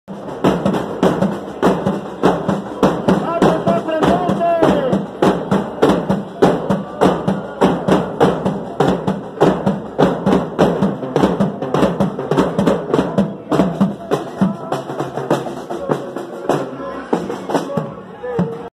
Gran convocatoria a la marcha del FUSSI (5/8/20)
Este miércoles por la mañana la Intersindical marchó por las calles céntricas y frente a la Legislatura Provincial.
Los gremios anunciaron un
Plan de Lucha donde todos los miércoles habrá marchas a la Legislatura para impedir La REFORMALABORAL
Consideran que allí es el ámbito donde hoy se encuentra el proyecto elevado por el ejecutivo